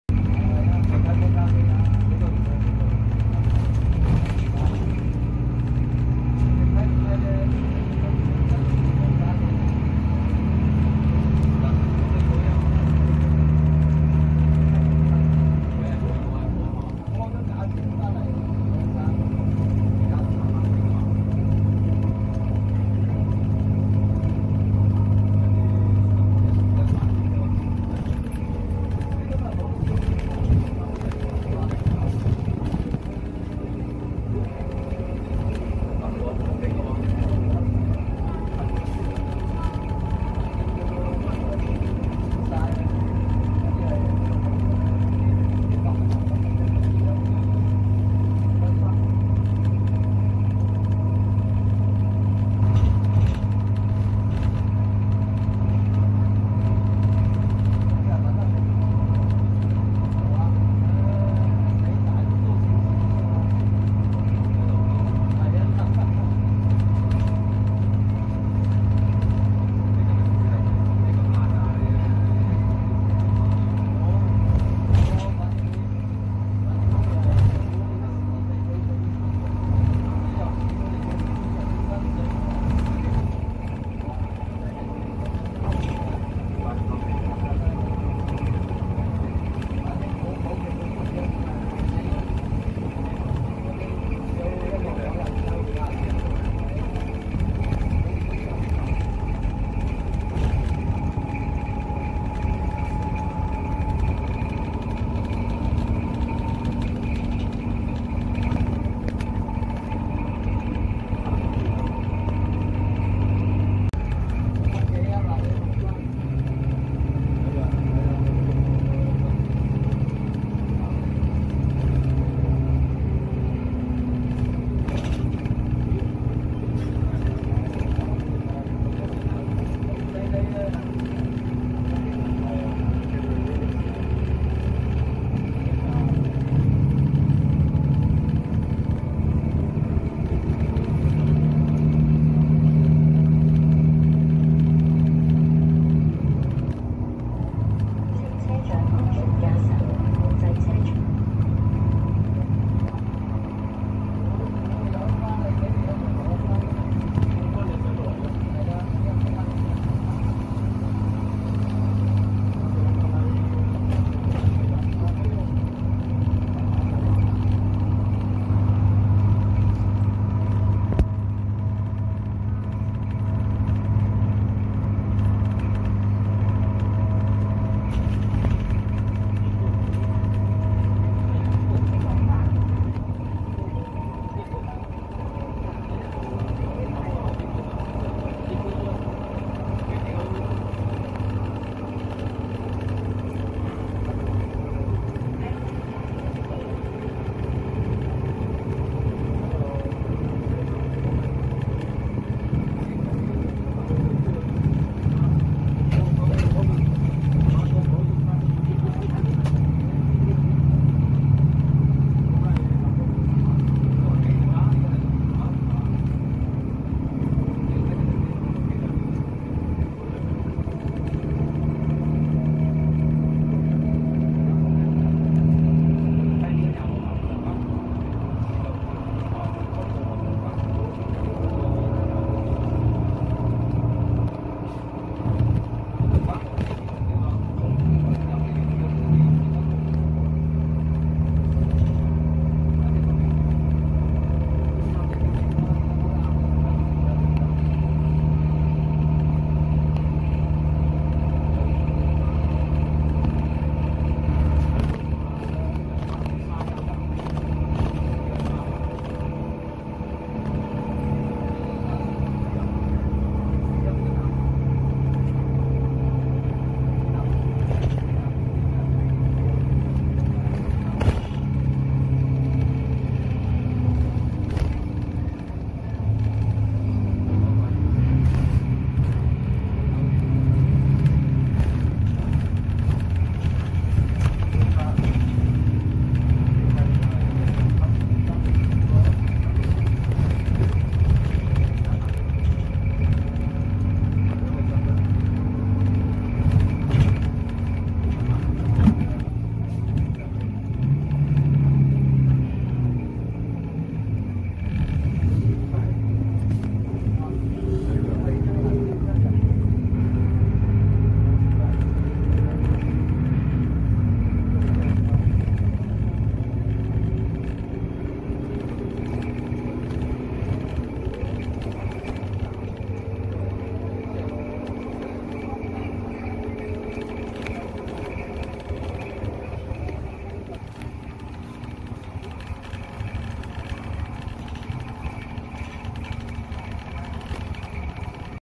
Zhuhai's Wanzi and Hengqin - a river away from Macau 珠海横琴及灣仔▪只離澳門一岸之隔
[影片播放位置]
00:17 對岸右手邊建築物所在地為「珠海横琴」
02:57 對岸右手邊建築物所在地為澳門地區

PS 原來02:57 左手邊的幾座建築物所在地才是「珠海灣仔」。字幕上誤解、敬請見諒。(29/7更新)

[影片簡介]
澳門西灣大橋與珠海横琴及灣仔相距不到一公里、可少有澳門人置業移居。

自費拍攝、旨在分享、非廣告内容。